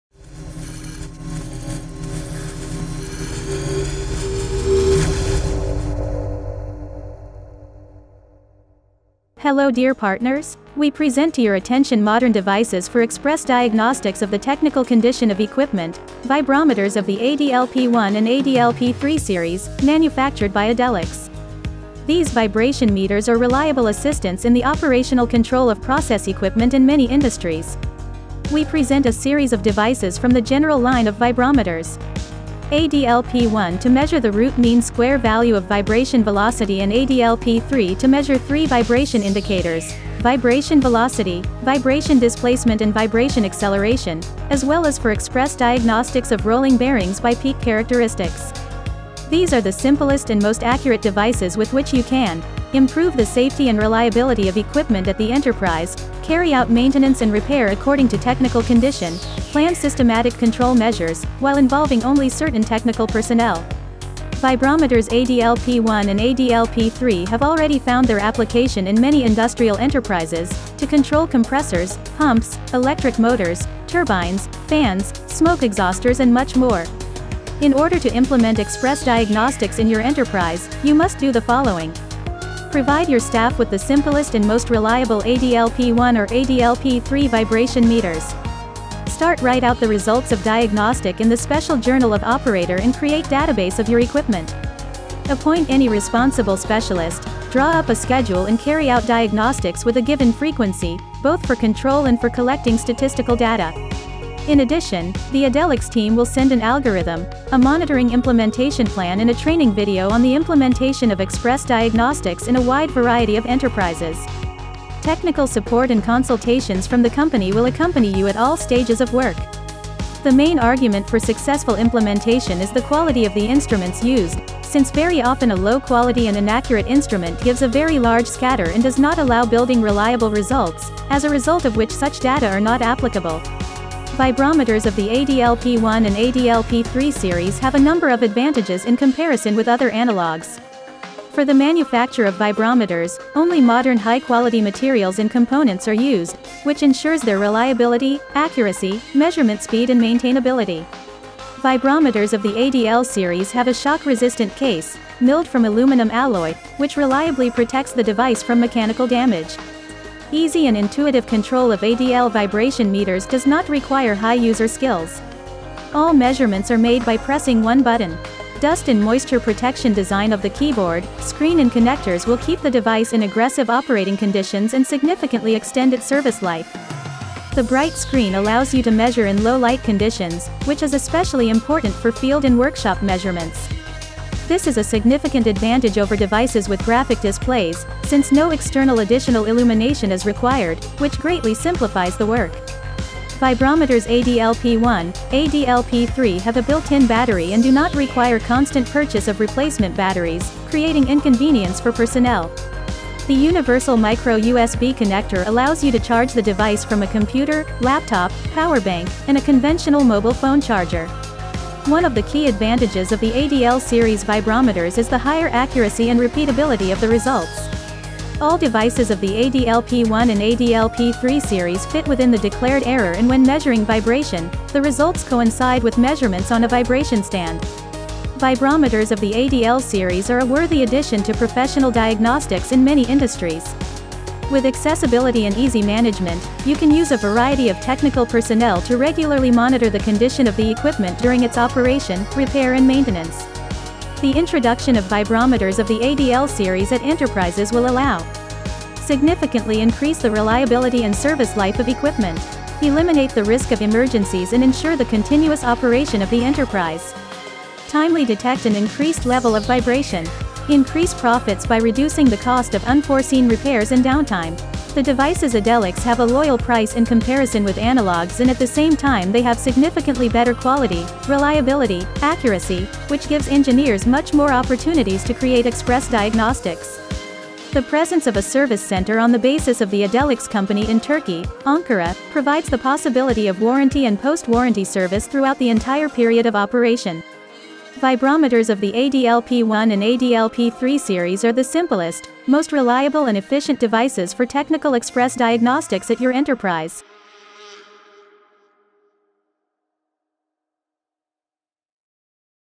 Universal vibrometers ADL P1/P3
We present to your attention modern devices for express diagnostics of the technical condition of equipment - Vibrometers of the ADL P1 and ADL P3 series, manufactured by Adelix.

ADL P1 - to measure the root-mean-square value of vibration velocity.

ADL P3 - to measure three vibration indicators: vibration velocity, vibration displacement and vibration acceleration, as well as for express diagnostics of rolling bearings by peak characteristics.

These are the simplest and most accurate devices with which you can: improve the safety and reliability of equipment at the enterprise, carry out maintenance and repair according to technical condition, plan systematic control measures, while involving only certain technical personnel.